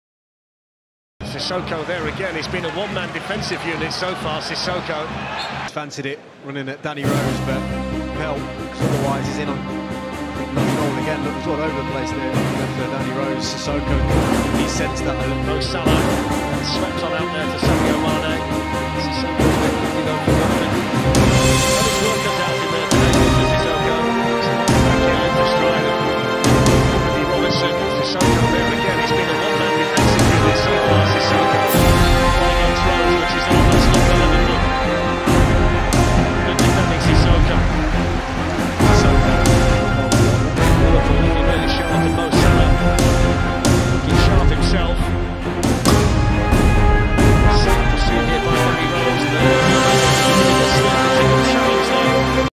Sissoko saved Danny Rose and Watford from further blushes against Liverpool
Watford's Moussa Sissoko saved his team and teammate Danny Rose from further blushes in their loss against Liverpool in the English Premier League EPL 21/22  match day 8.